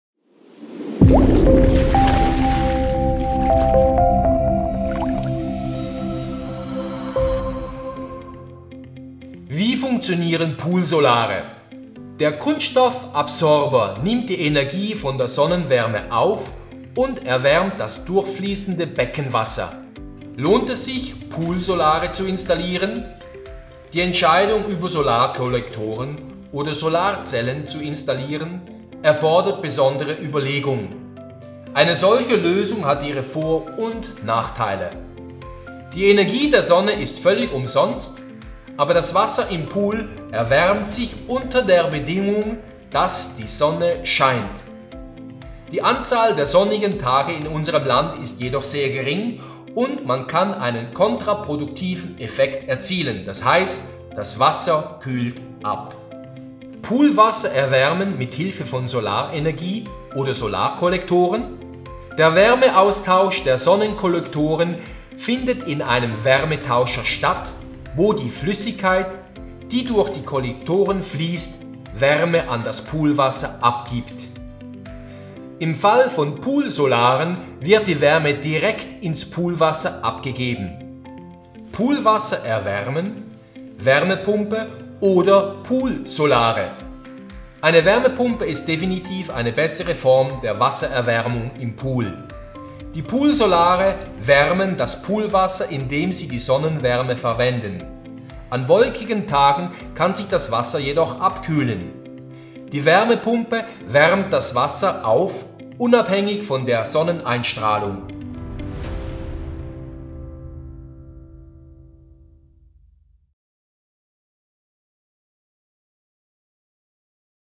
21. PoolsFACTORY GROUP GFK Pool - Poolheizung, welches Heizsystem ist am besten geeignet
Der Film beantwortet Fragen:
1. Wie funktionieren Pool-Solare?
2. Lohnt es sich, Pool-Solare zu installieren?
3. Poolwasser erwärmen mit Hilfe von Solarenergie / Solarkollektoren.
4. Poolwasser erwärmen: Wärmepumpe oder Pool-Solare?